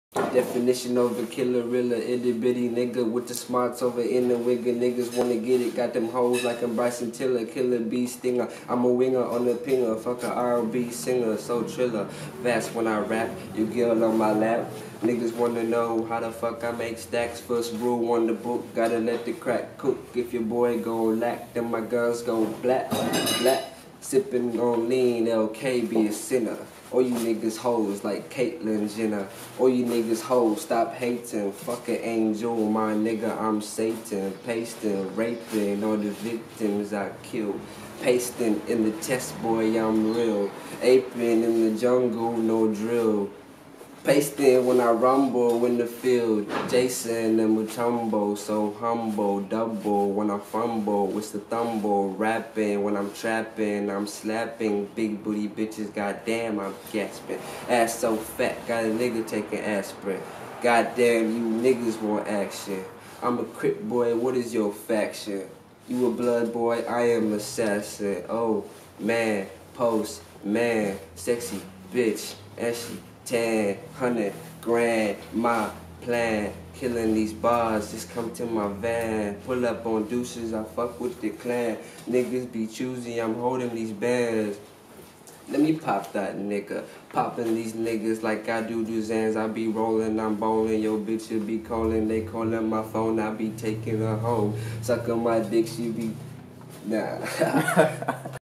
Perth rapper K.Mass freestyle WAVY TRAX
WAVY TRAX Friday Freestyle presents K.MASS upcoming rapper from Perth Western Australia